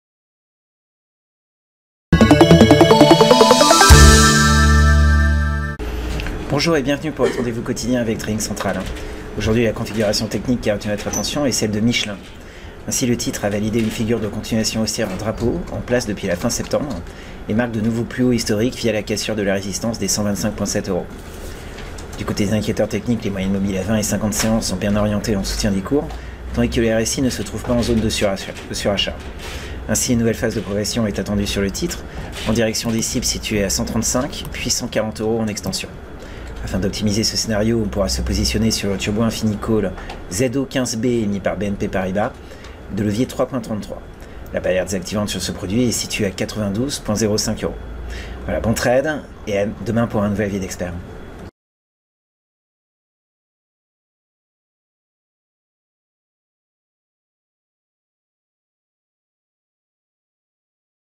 Avis d’Expert Michelin: Turbo Infini Call ZO15B
Aujourd'hui, la configuration technique qui a retenu notre attention est celle de Michelin.